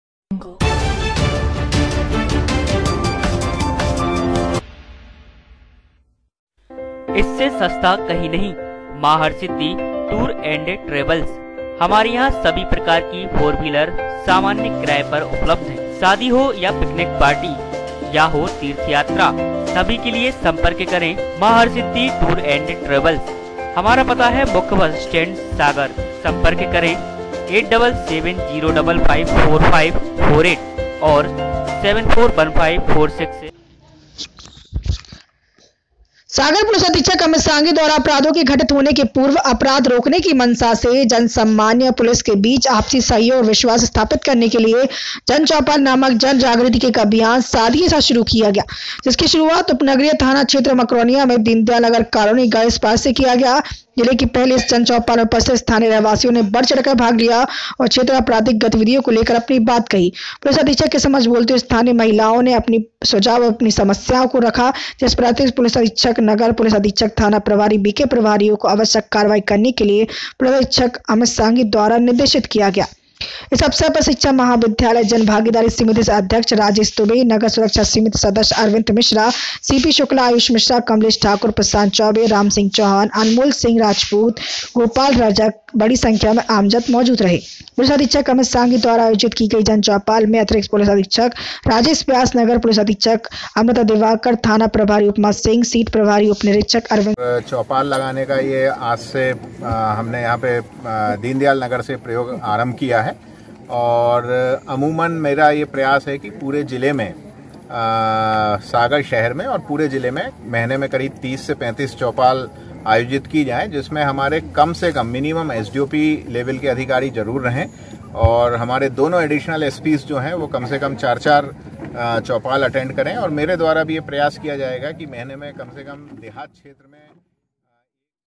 सागर पुलिस अधीक्षक अमित सांघी द्वारा अपराधों के घटित होने के पूर्व अपराध रोकने की मंशा से जन सामान्य
सागर पुलिस अधीक्षक अमित सांघी द्वारा अपराधों के घटित होने के पूर्व अपराध रोकने की मंशा से जन सामान्य और पुलिस के बीच आपसी सहयोग और बिश्वास स्थापित करने के लिए जन चौपाल नामक जन